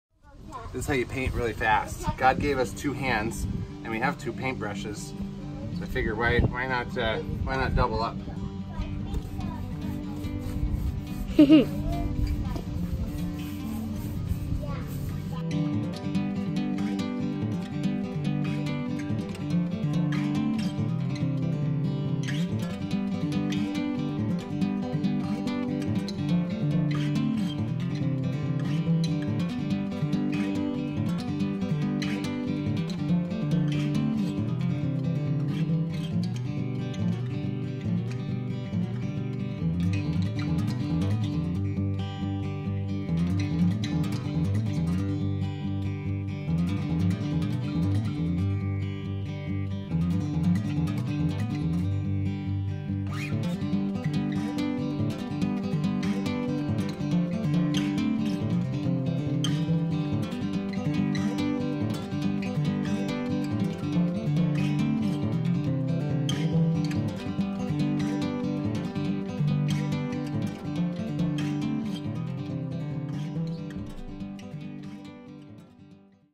Fast deck painting
How to paint a deck really fast ... use two hands and two paint brushes!